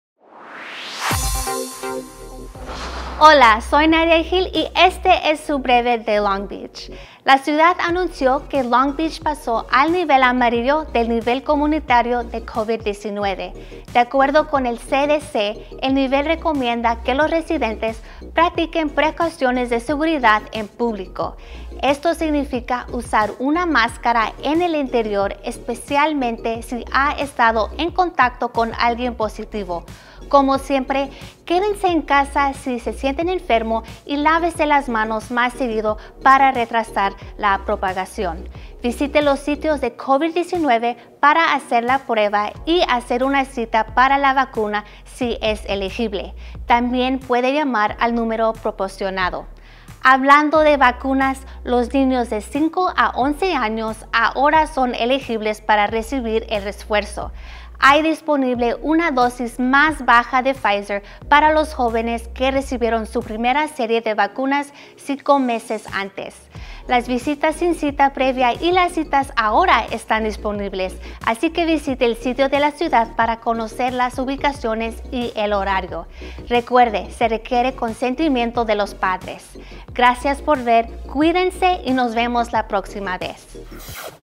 Long Beach Brief, 30 de mayo de 2022
Long Beach Brief es su información semanal de eventos e información en la ciudad de Long Beach.  Esta semana presentamos:

El Nivel Amarillo del Nivel Comunitario de COVID-19 y refuerzos para jovenes de edades 5-11.